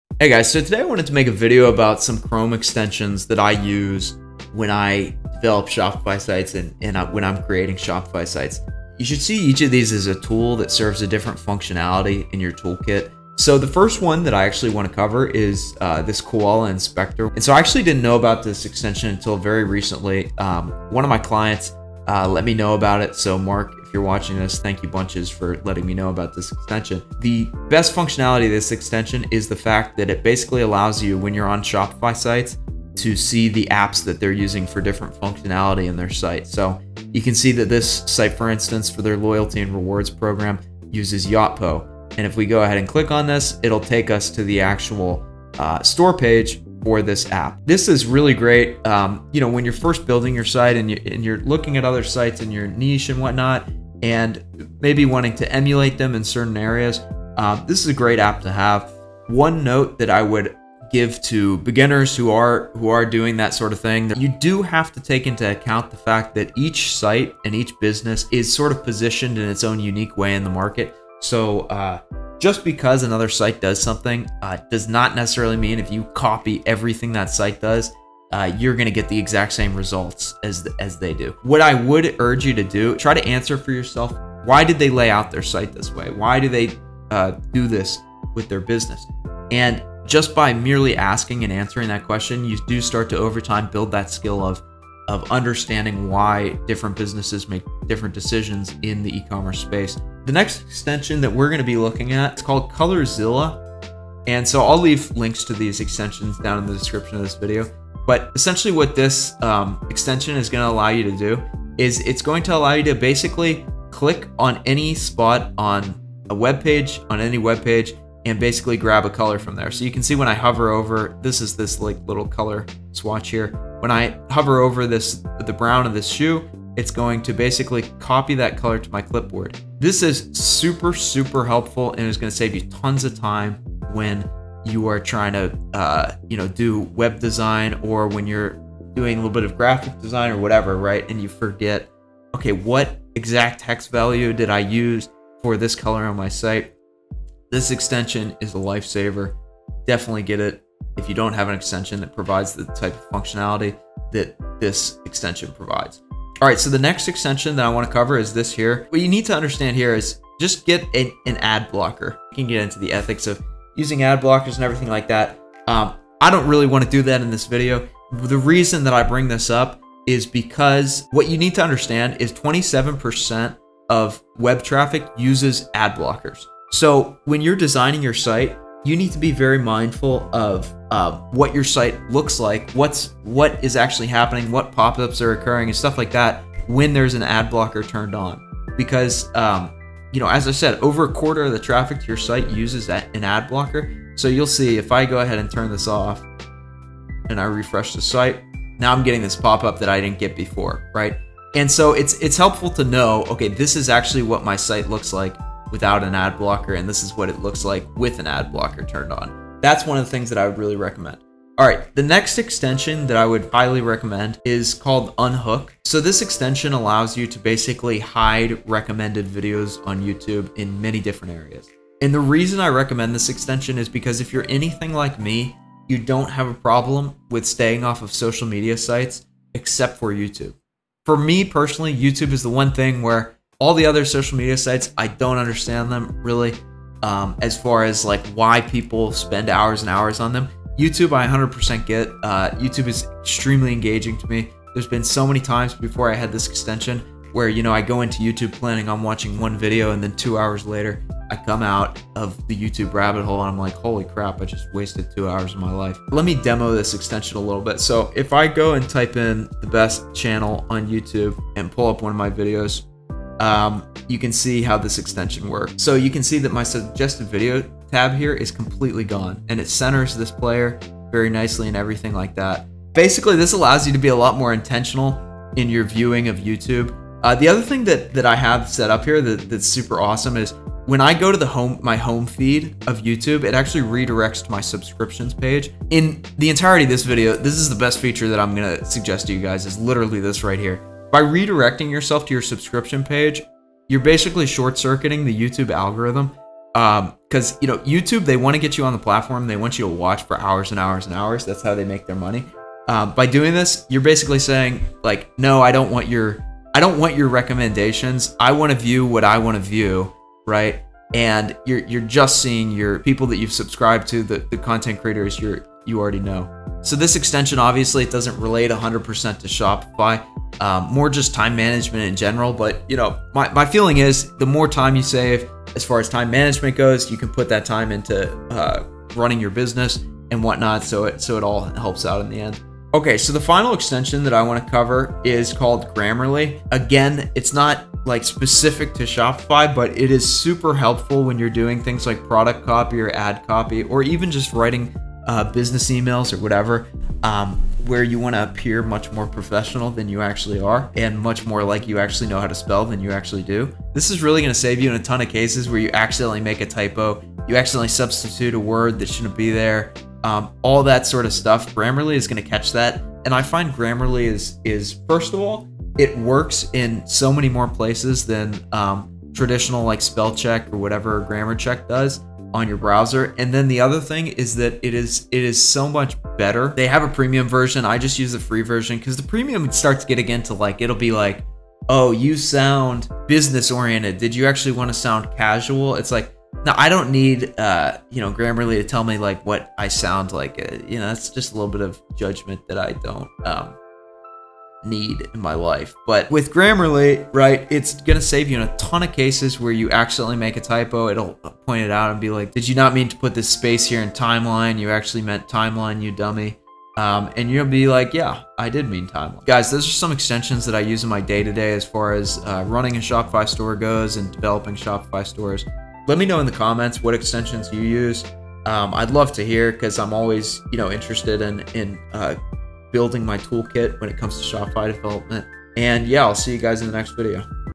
Best Shopify Browser Extensions
In this video, I cover my favorite browser extensions for creating a Shopify site.

Timestamps:
0:00 Intro
0:15 Koala Inspector
1:40 Colorzilla
2:37 uBlock Origin
3:32 Unhook
5:33 Grammarly